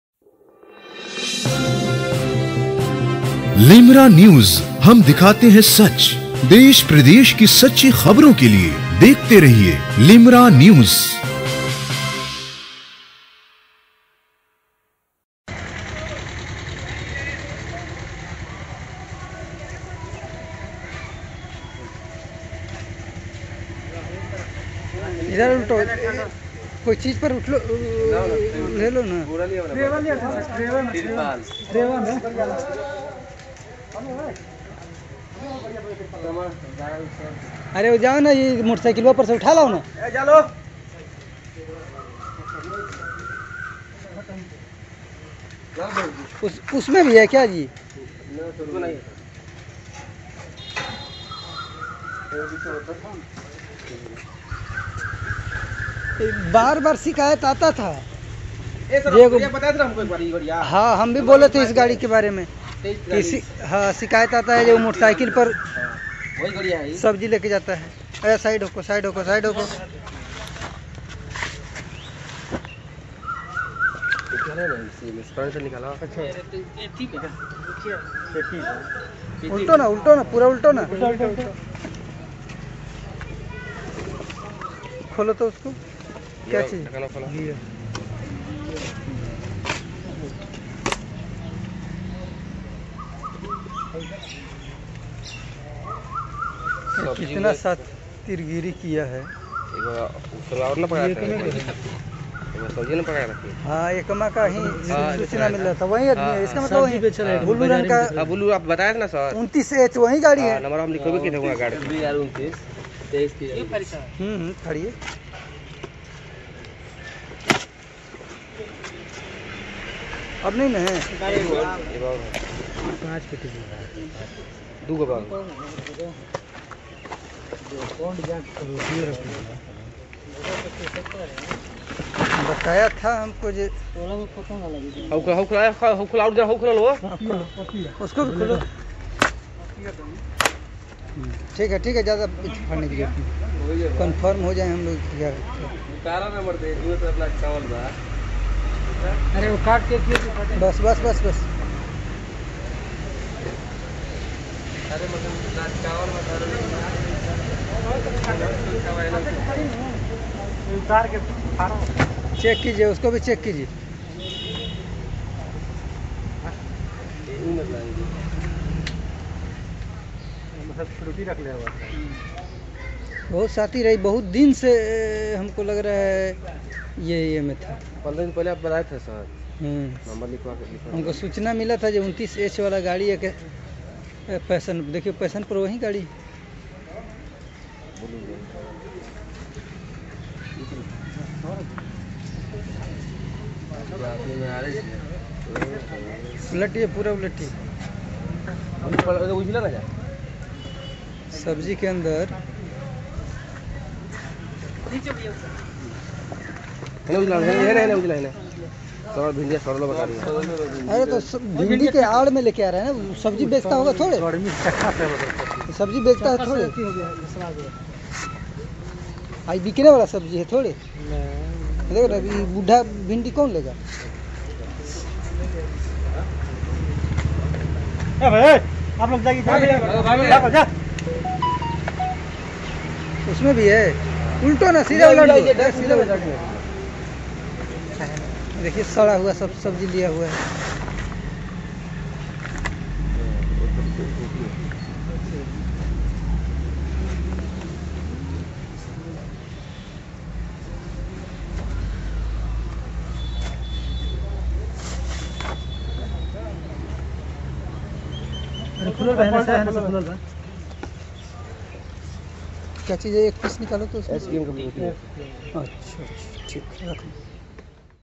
भिंडी की बोरी से बरामद हुई शराब-Chapra-Bihar-Limra News
आप अपनी न्यूज़ हम तक पहुंचा सकते हैं.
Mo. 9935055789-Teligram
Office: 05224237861

Producer: MD Naushad Bilgrami
Editor: Adnan Khan